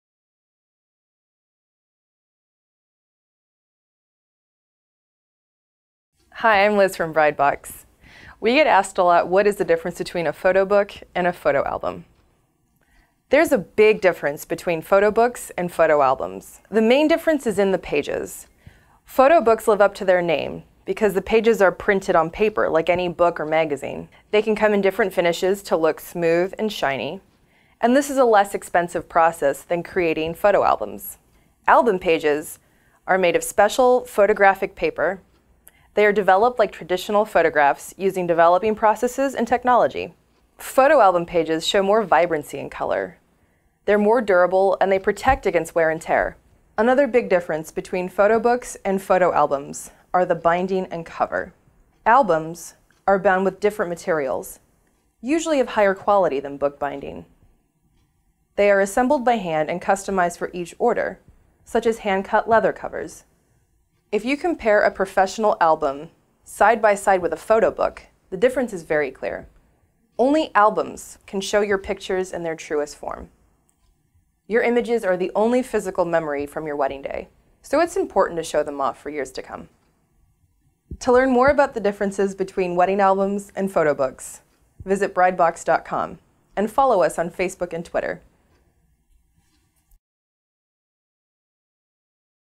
Difference Between Wedding Album And Photo Book
Between the covers and the page types, there are several differences between a wedding album and photo book.